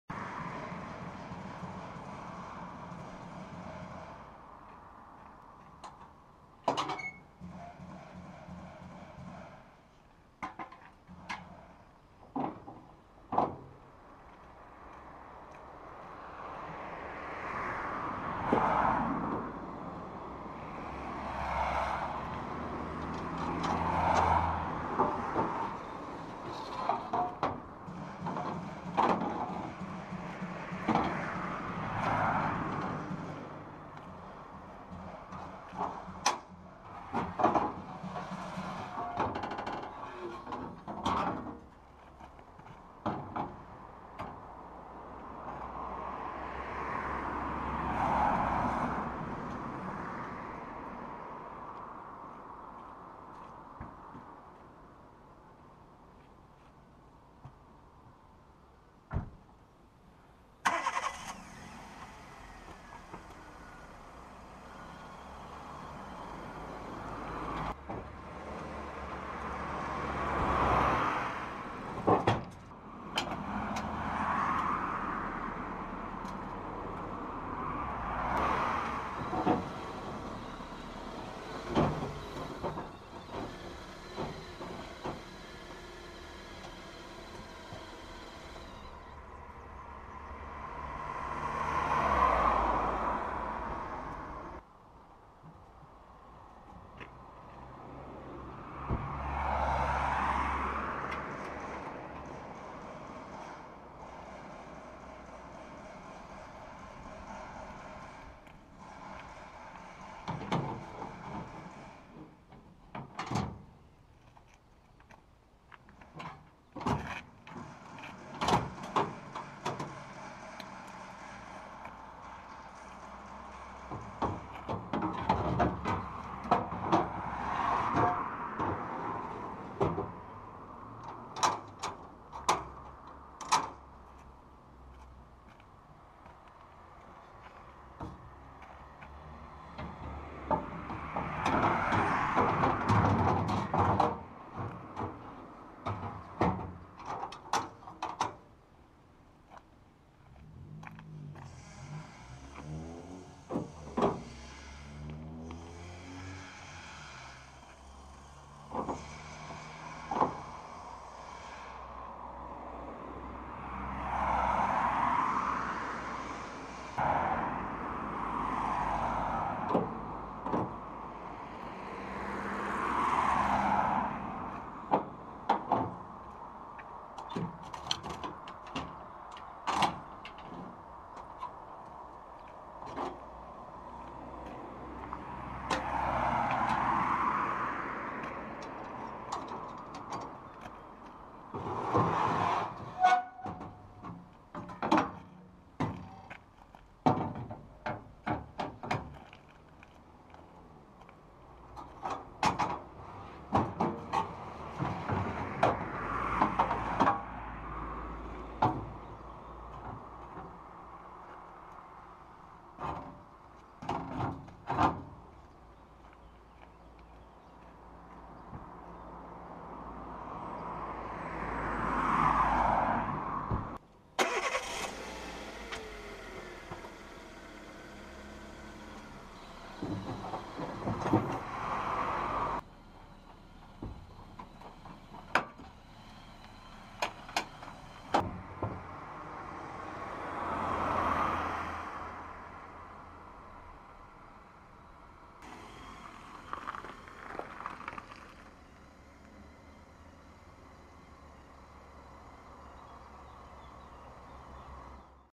Utilisation d'une remorque Anssems AMT 2500
Utilisation d'une remorque porte voiture modèle AMT2500
en vente et en location chez Remorques RM à Uchaud.